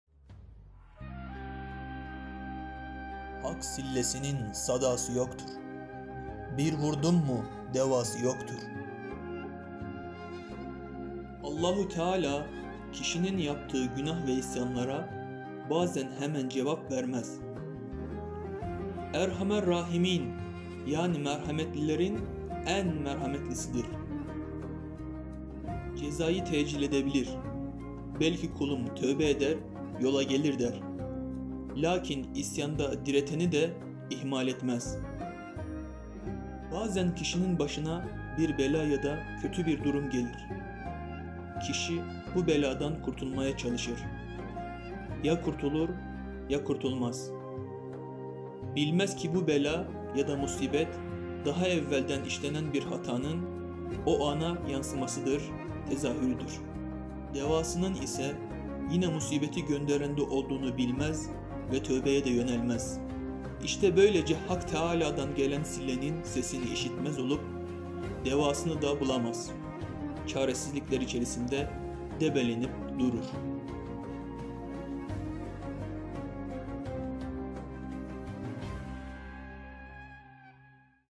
Hak Sillesinin Sedası Yoktur Bir Vurdum Mu Devası Yoktur-Yeni Moda Sözler
Hak Sillesinin Sedası Yoktur
Bir Vurdum Mu Devası Yoktur

Allah-u Teala kişinin yaptığı günah ve isyanlara bazen hemen ceza vermez. Erhamerrahimin yani merhametlilerin en merhametlisidir. Cezayı tecil edebilir belki kulum tövbe eder yola gelir der, lakin isyanda direteni de ihmal etmez. Bazen kişinin başına bir bela ya da kötü bir durum gelir. Kişi bu beladan kurtulmaya çalışır. Ya kurtulur ya kurtulmaz. Bilmez ki bu bela yada musibet daha evvelden işlenen bir hatanın o ana yansıyan tezahürüdür. Devasının ise yine musibeti gönderenden olduğunu bilmez ve tövbeye de yönelmez. İşte böylece Hak Tealadan gelen sillenin sesini işitemez olup devasını da bulamaz; debelenip durur.